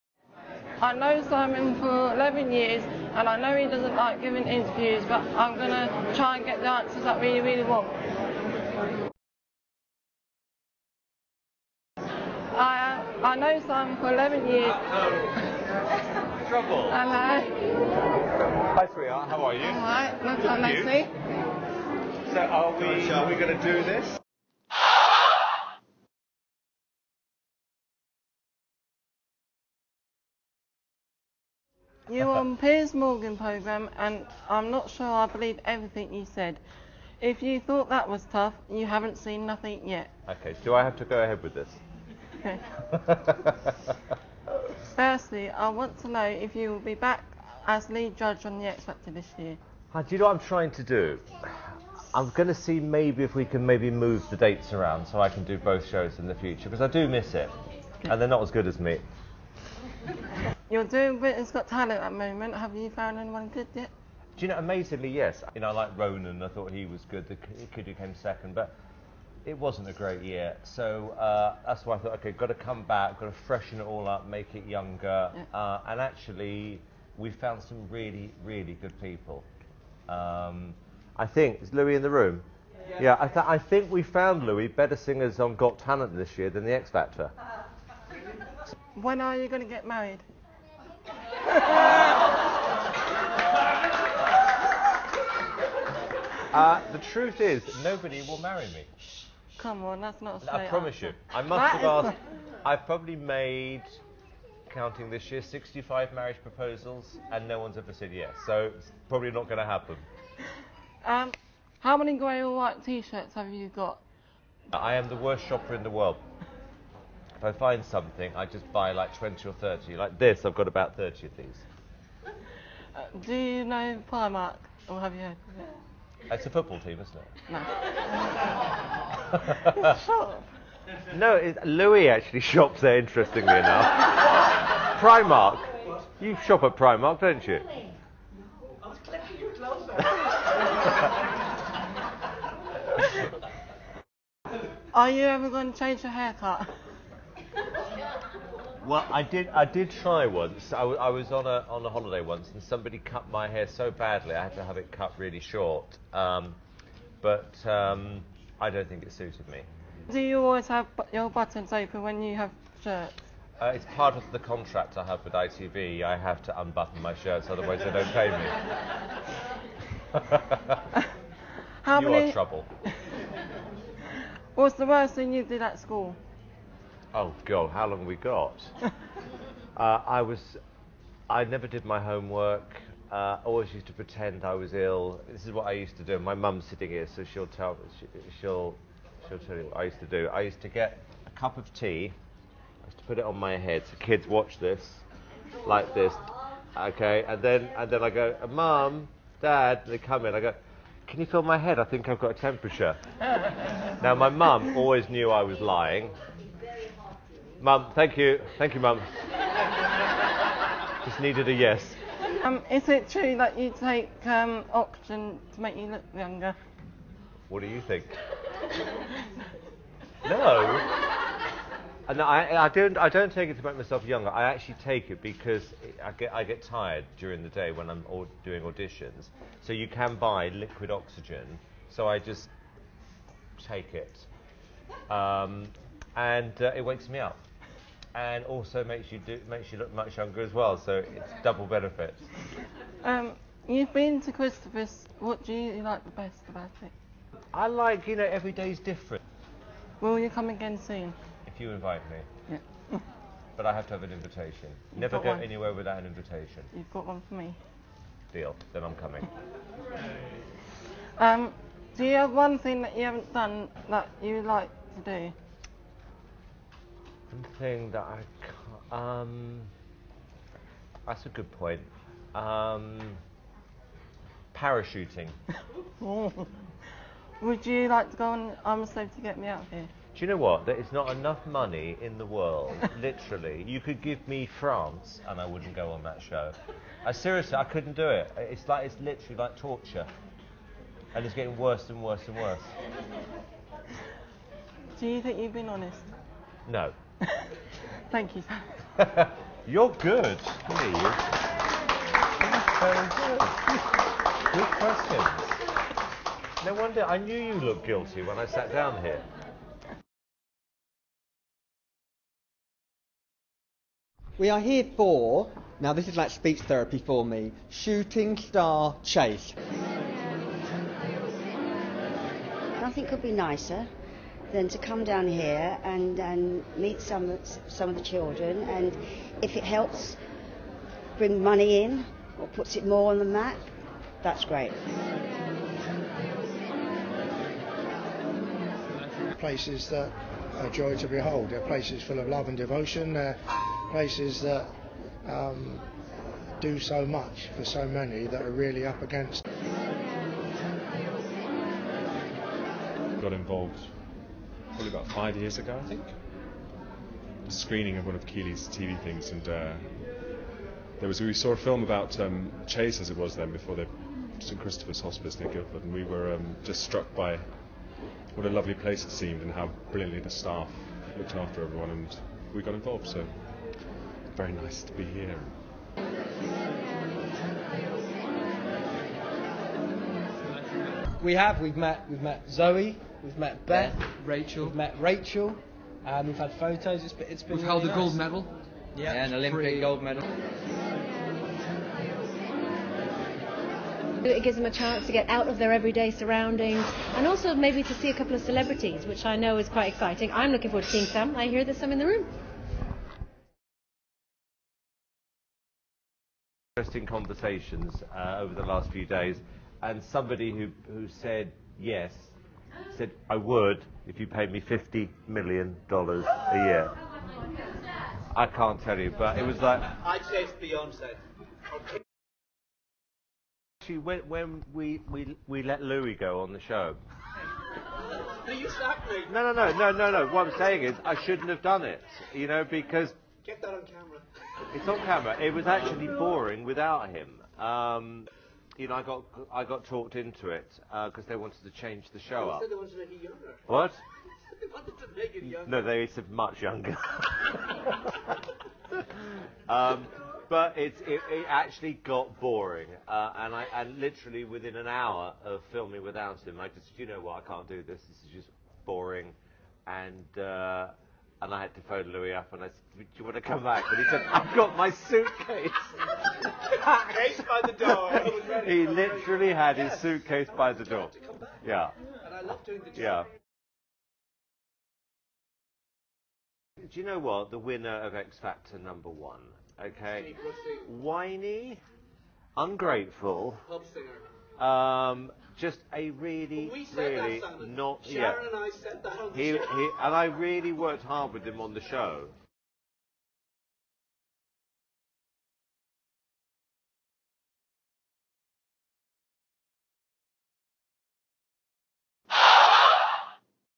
Simon Cowell talks to Shooting Star CHASE
Children and teenagers supported by Shooting Star CHASE put Simon Cowell in the hot seat.